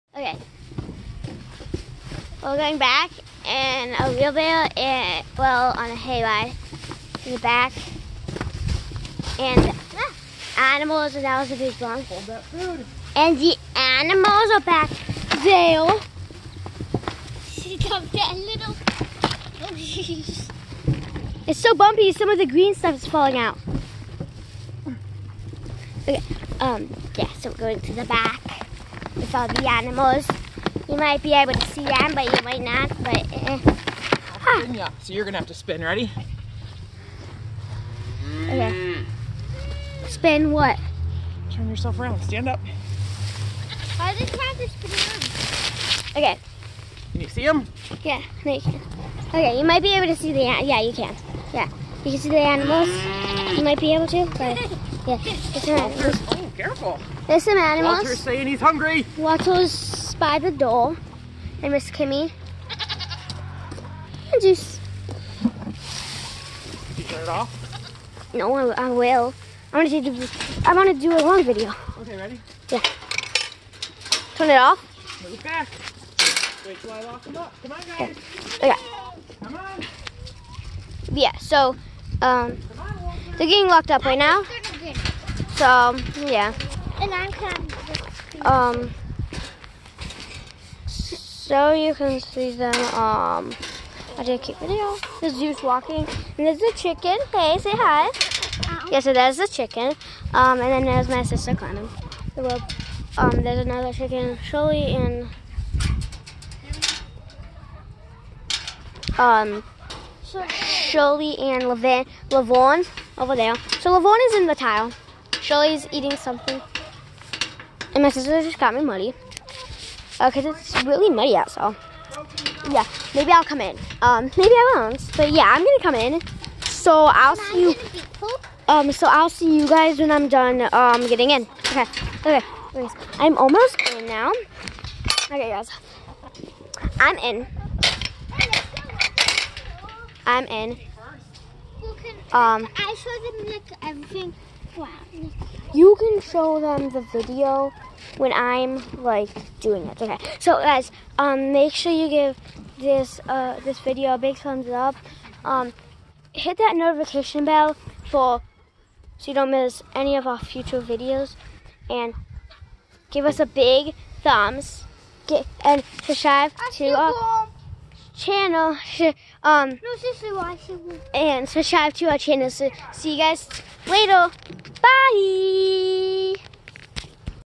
Nighttime routine hay ride
Since we have been feeding hay off of a round bale we have to take it back on rhe wheelbarrow, kids make perfect weights so we don't lose any along the path 😂😂 Follow us on Facebook Instagram and Twitter @whfarmchardon Shop my farm store at whfarmchardon.com